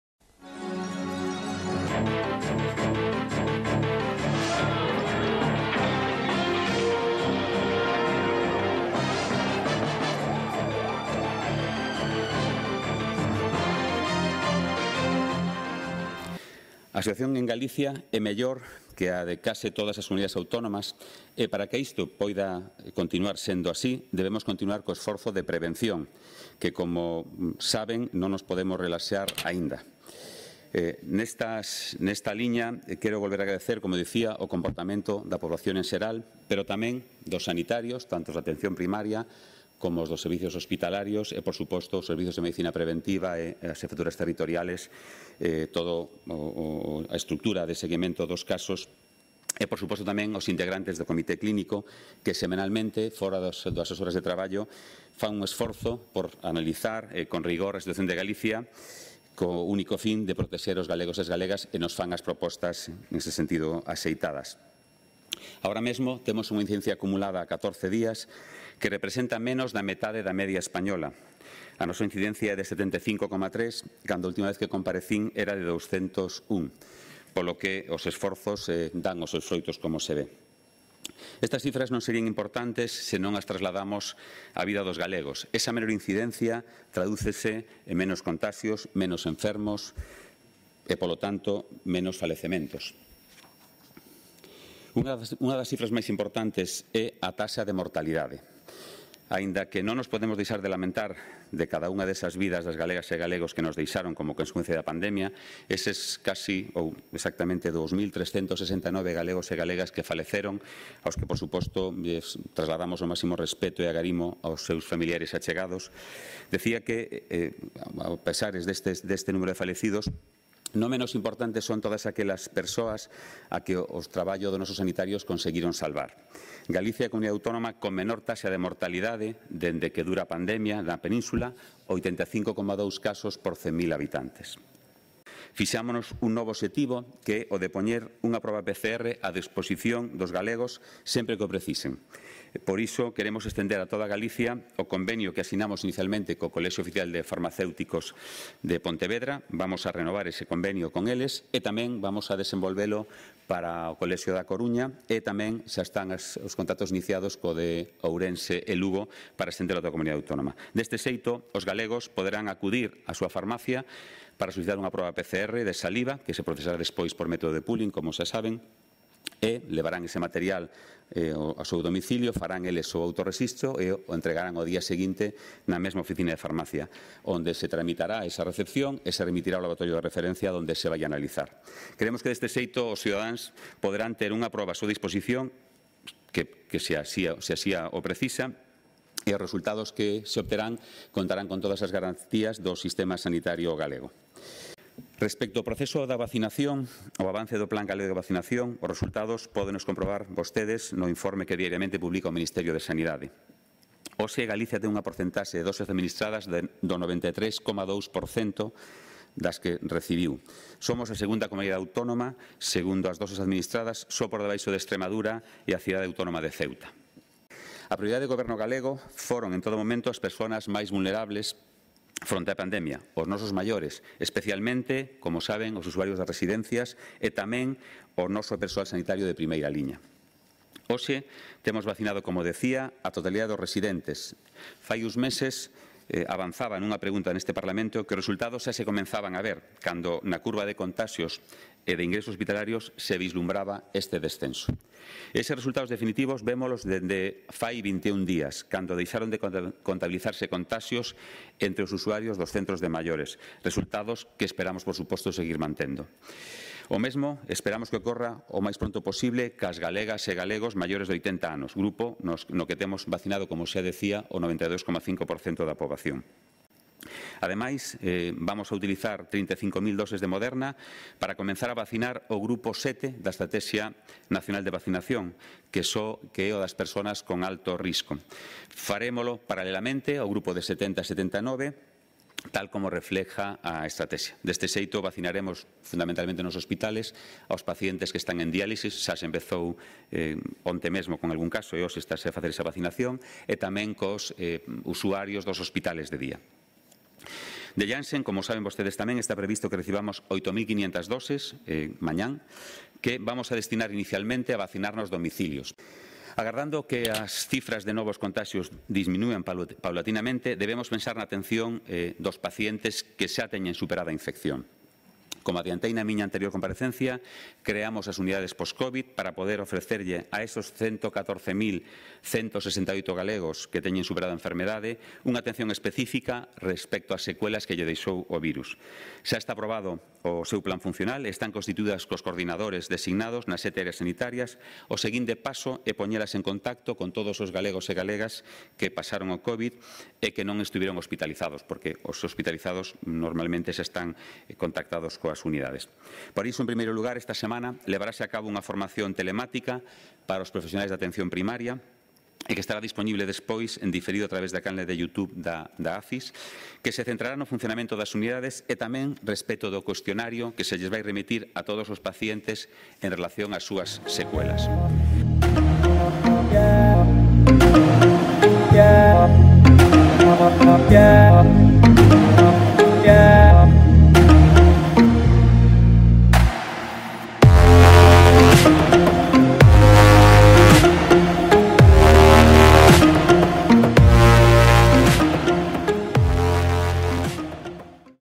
A Xunta estima ter vacinado a preto do 30% da poboación ao remate desta semana
13 de abril de 2021
O conselleiro de Sanidade, Julio García Comesaña, compareceu hoxe, a petición propia, no Parlamento de Galicia, para informar sobre a evolución da incidencia da pandemia en Galicia no relativo á situación epidemiolóxica e asistencial e aos avances no Plan galego de vacinación fronte á covid.

Tal e como subliñou o conselleiro, grazas á previsión na organización e loxística no desenvolvemento do Plan galego de vacinación, o domingo pasado a Xunta tiña vacinadas 546.630 persoas, o 23,1% da poboación. Esta semana prevense administrar 138.500 vacinas, polo que se remataría cunha estimación de 685.130 vacinas, que sería xa un 29% do total da poboación a vacinar, -remarcou o conselleiro-.

Na súa intervención, García Comesaña destacou que Galicia ten unha porcentaxe de vacinación de doses administradas do 93,2%, o que a sitúa na segunda comunidade do Estado neste apartado.

Anunciou o comezou da vacinación de persoas con moi alto risco, ao que irán destinadas 35.000 doses de Moderna, e sinalou que, da vacina de Janssen, está previsto que se reciban mañá 8.500 doses. Estas últimas, destinaranse á vacinación nos domicilios. De AstraZeneca está prevista a chegada de 7.500 vacinas, que permitirá levar a cabo unha vacinación masiva de 15.000 persoas do grupo de 60 a 65 anos, a vindeira fin de semana.

Iniciouse, -dixo tamén-, a vacinación a domicilio dos grandes dependentes e dos maiores de 80 anos que non puideron achegarse aos centros de saúde. Paralelamente, séguense a vacinar aos grupos de idade de 75 a 79 e aos de 60 a 69. Neste apartado, o conselleiro remarcou que, no grupo de persoas maiores de 80 anos, xa se vacinou ao 92,5%.

O conselleiro remarcou que, o seu departamento, ten previsto que todas as áreas sanitarias teñan organizados as quendas de enfermería cunha programación semanal para que, no caso de recibirse un maior número de viais, se poida convocar unha vacinación masiva o máis axiña posible.

Ademais, tamén anunciou que a Xunta vai poñer, proximamente, a disposición da cidadanía, unha páxina web na que se poidan consultar todos os datos referidos ao proceso de vacinación, incluídas as campañas en marcha e os grupos de idade aos que afecta.